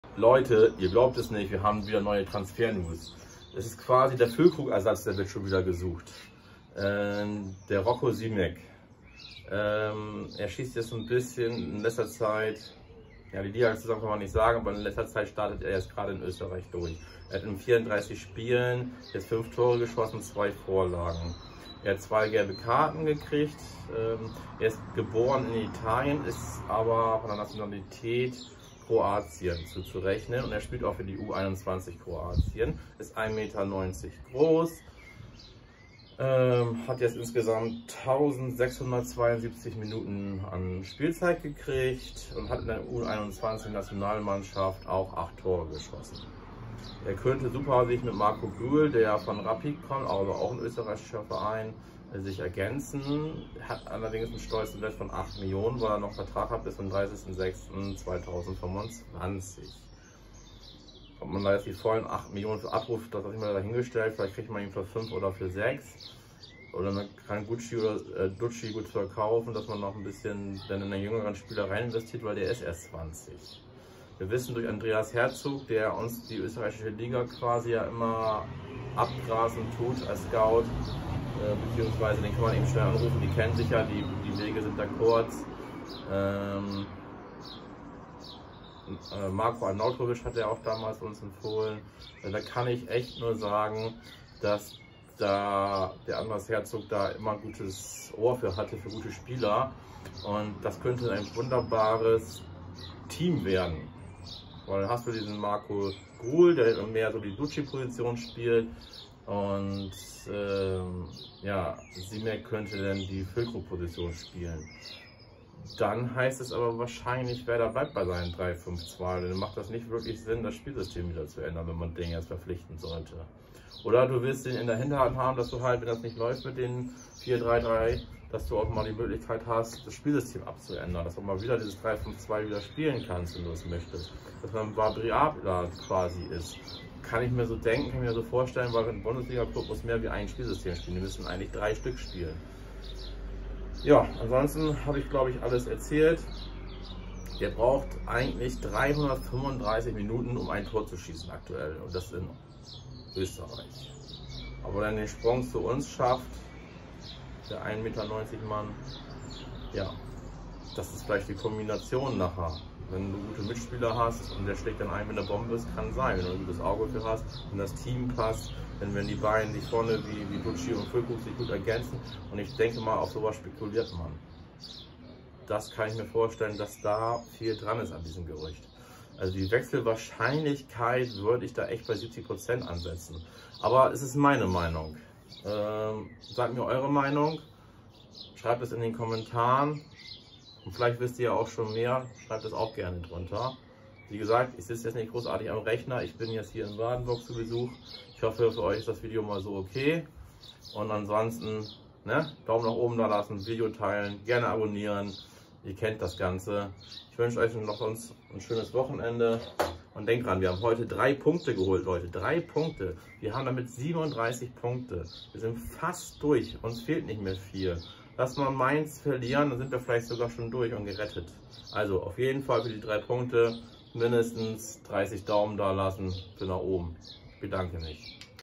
Transfernews Salzburgs Mittelstürmer Roko Simic soll vor einer Verpflichtung zu Werder Bremen stehen
Salzburgs Roko Simic. Er soll im Dreiersturm den Mittelstürmer geben